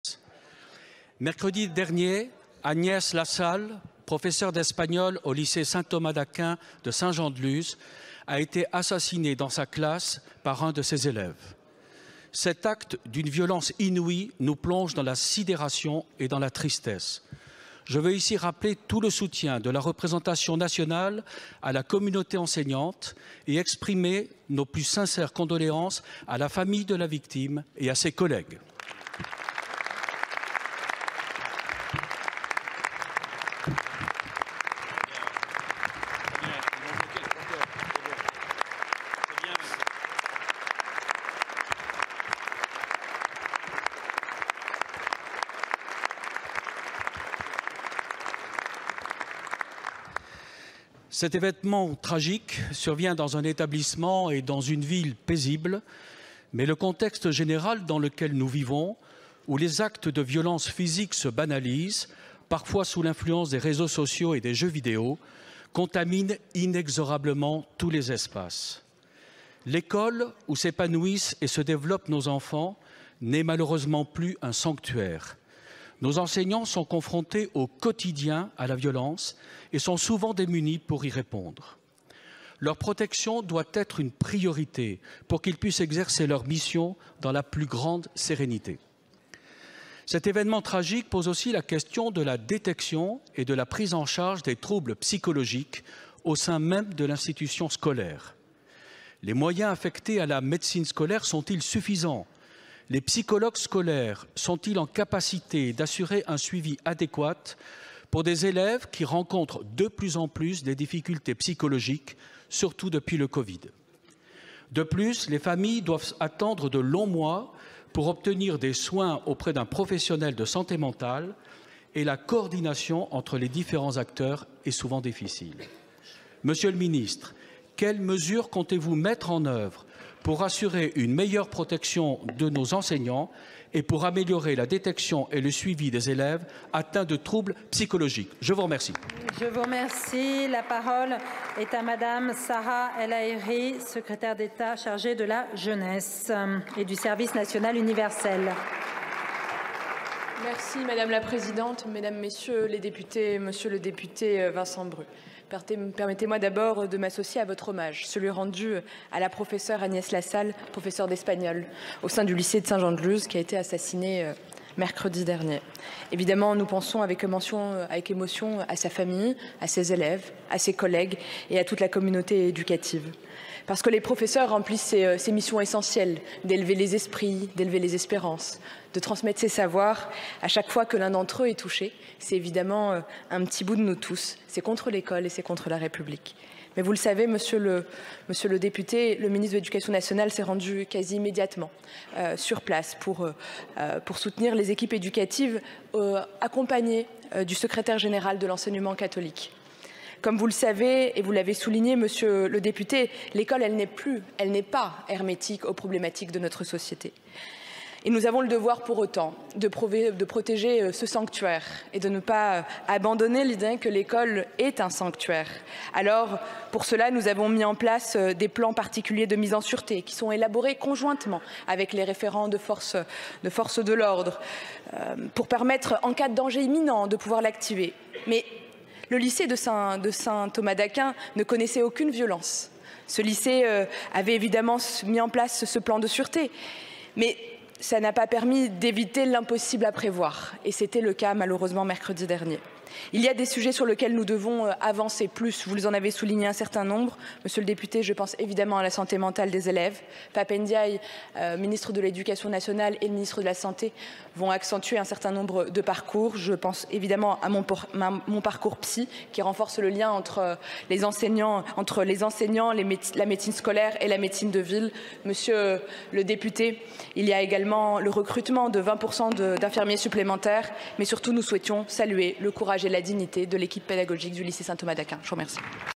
Assassinat d'Agnès Lassalle au Lycée Saint Thomas d'Aquin
Vincent Bru, Député des Pyrénées-Atlantiques, a interrogé Sarah El Haïry, Secrétaire d'État à la Jeunesse et au SNU, suite à l'assassinat d'Agnès Lassalle, professeure d'espagnol au Lycée Saint-Thomas d'Aquin. Revoir la QAG.